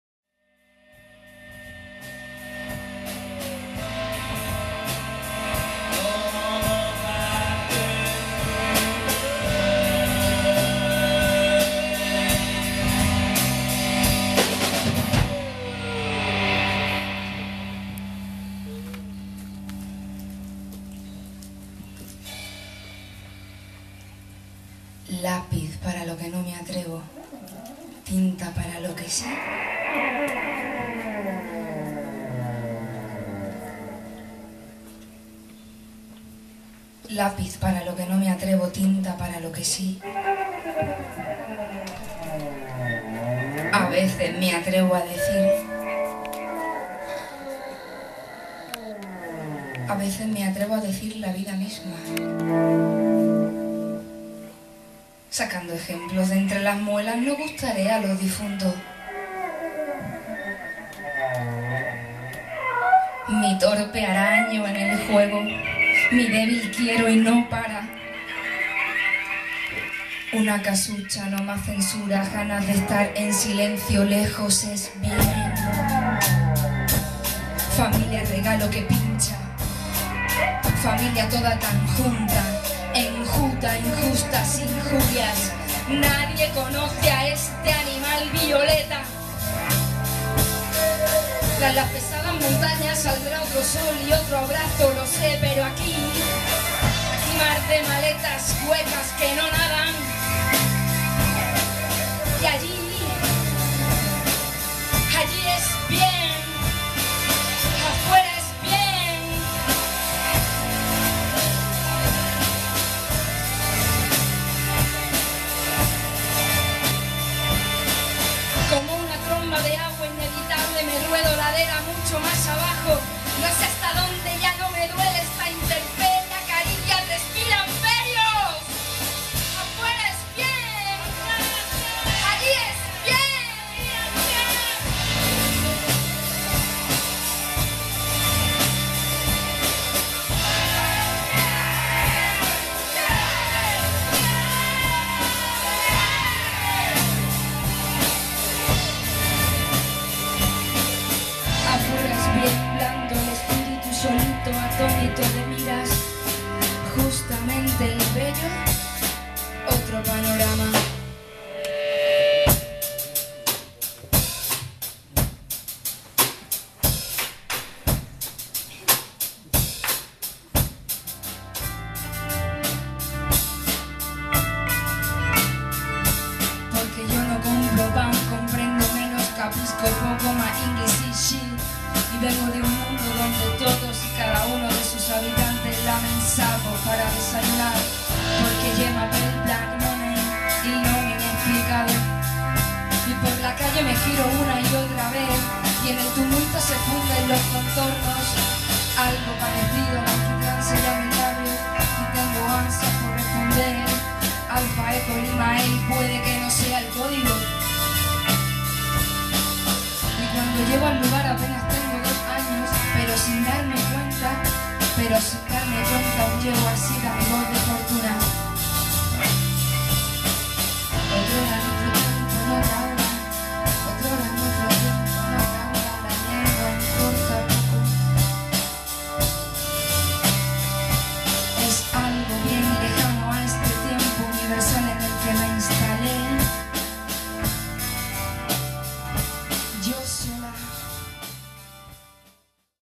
Marta Mapache & A Hombros de Enanos
Fragmento de la actuación de Marta Mapache & A Hombros de Enanos. CICUS. Febrero 2012.